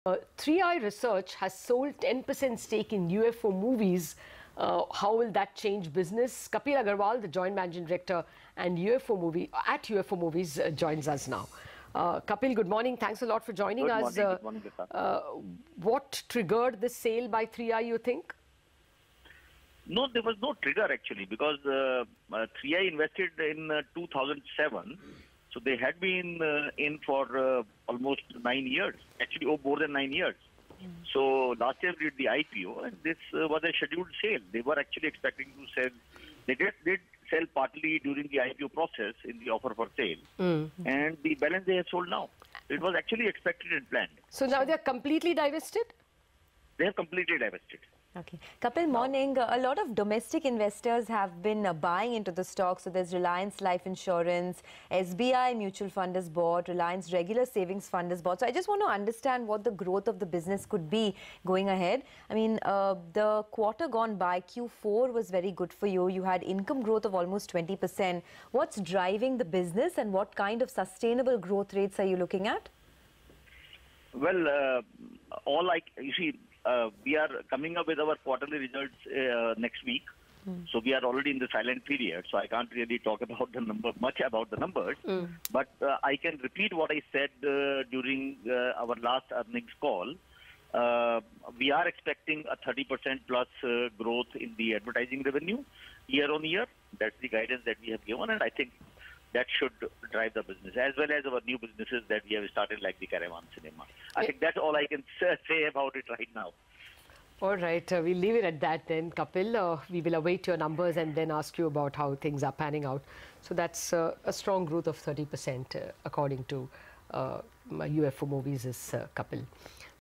Stake Sale By 3i Research Was On Expected Lines: UFO Moviez
3i Research has sold 10 per cent stake in UFO Moviez. Kapil Agarwal, Joint MD, UFO Moviez speaks to CNBC-TV18 and states that stake sale by 3i was on expected lines and that 3i has completely exited the company now.